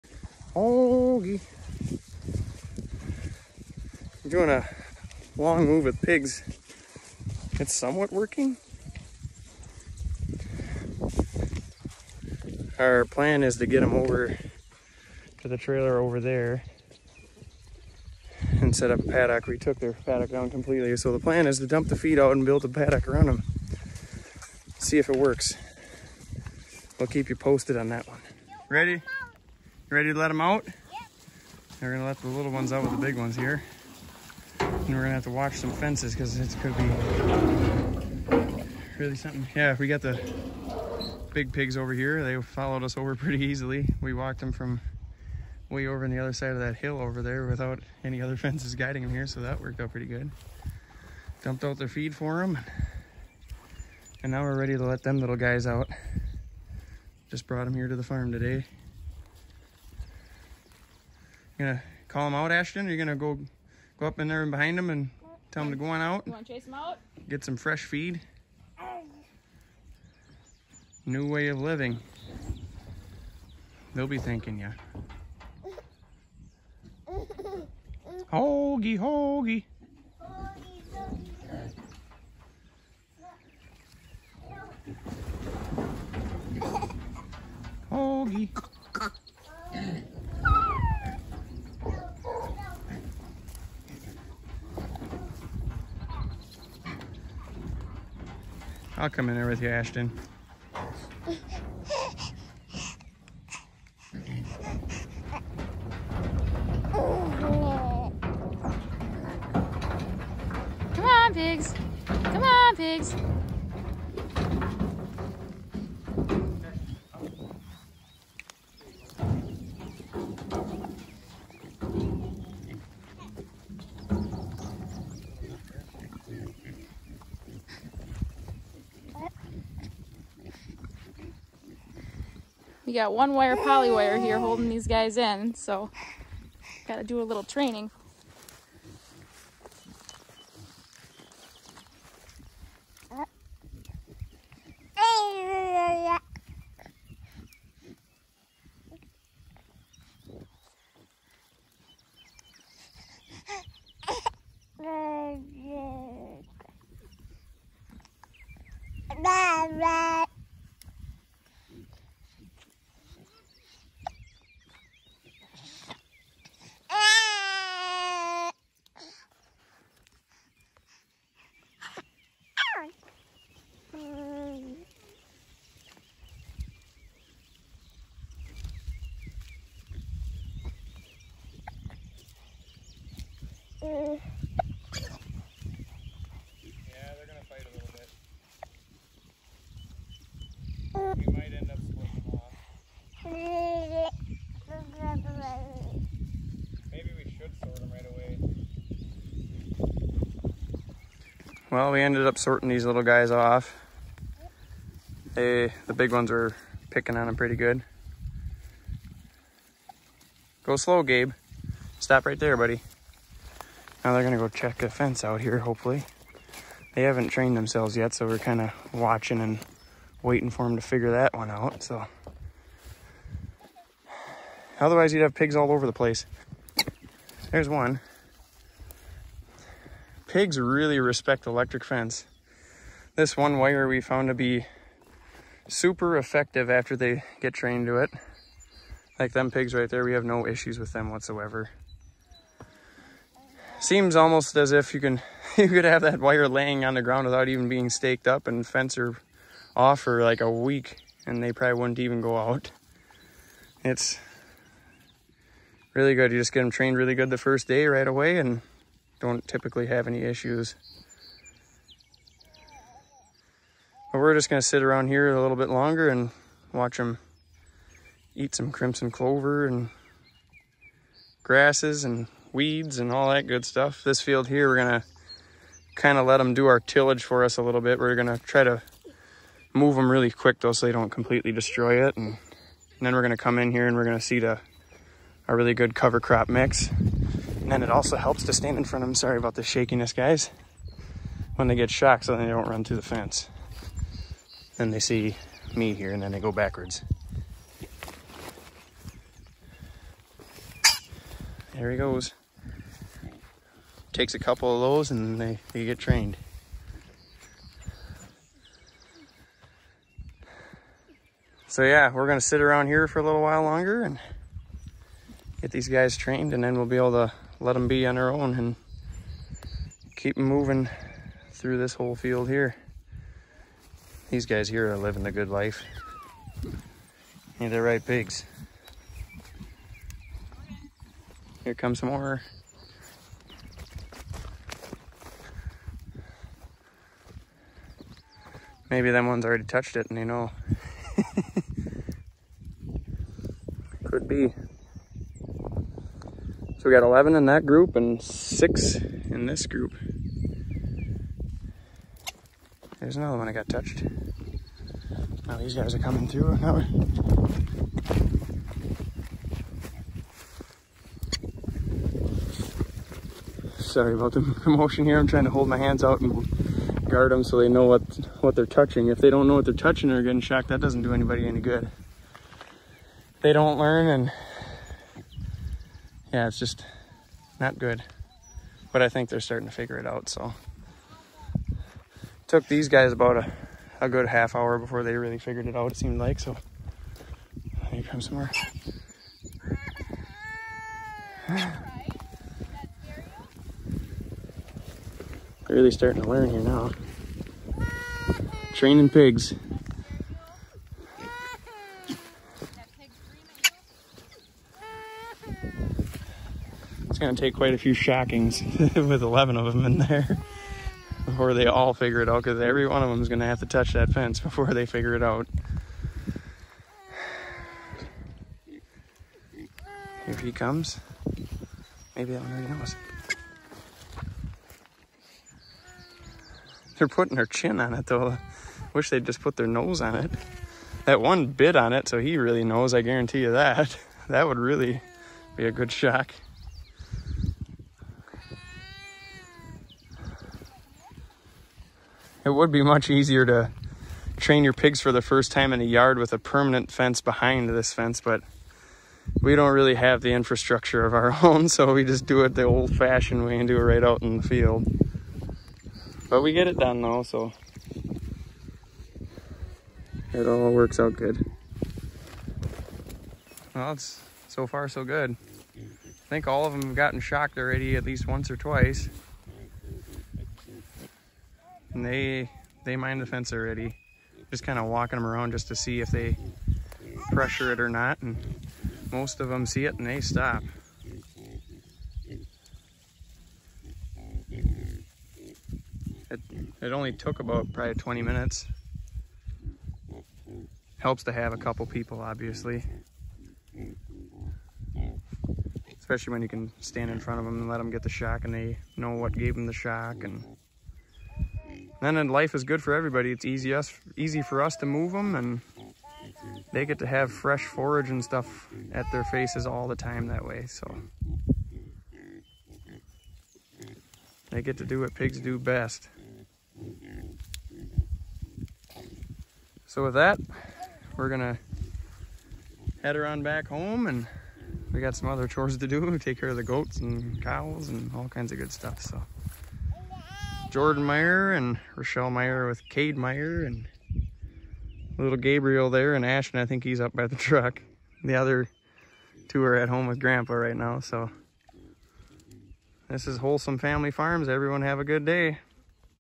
Training pasture raised pigs to single electric wire
Moving the pigs to a new paddock and training our new group of pigs to single wire electric fence.